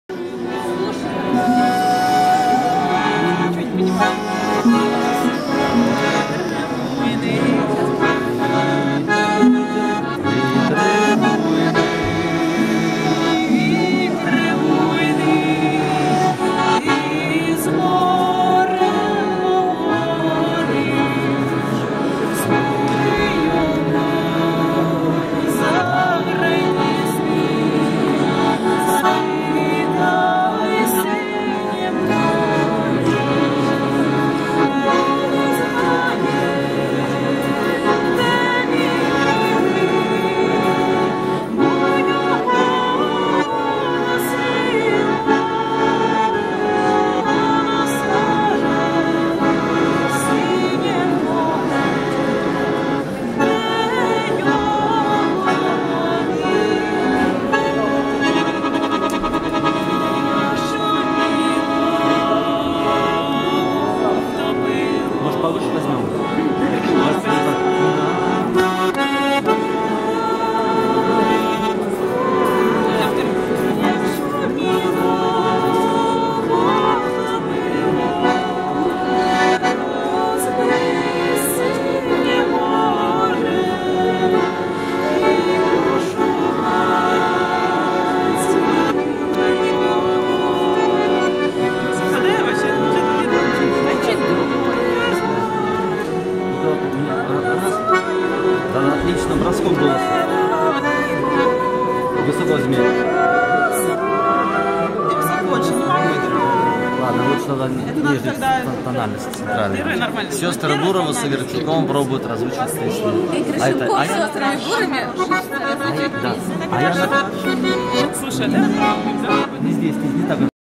Февральские встречи в Москве. Часть 2
Игорь Шипков и сёстры Буровы в кулуарах юбилейных съёмок программы "Играй, гармонь!"
Москва, 23 февраля 2016 года.